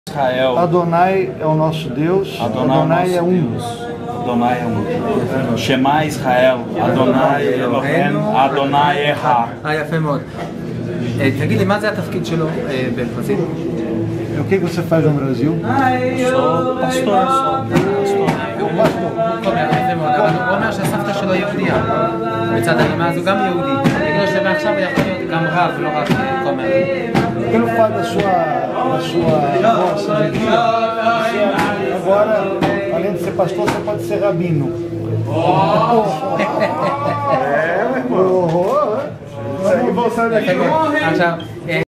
כומר בקבר דוד מתרגש לגלות שהוא יהודי
בקבר דוד יש אדם ששואל תיירים אם אימא שלהם או סבתא מצד אימא שלהם יהודיה. הרבה תיירים לא יודעים שאם כן אז זה אומר שגם הם יהודים... כשהם מגלים שהם יהודים הוא מניח להם תפילין..